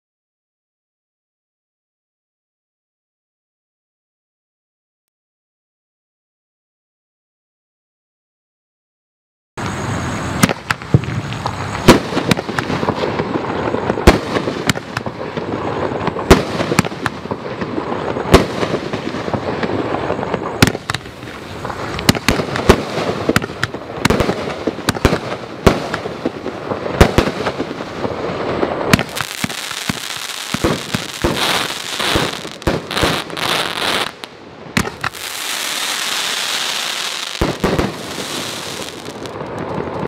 Lady Liberty TS5656
500 Gram 21 Shots

Description: Check this cake with V shaped nonstop shooting red white and blue, the best combination of ti-gold palm tree with Cyan dahlia! Also a double finale of silver silk tail to flower willow with sky blue!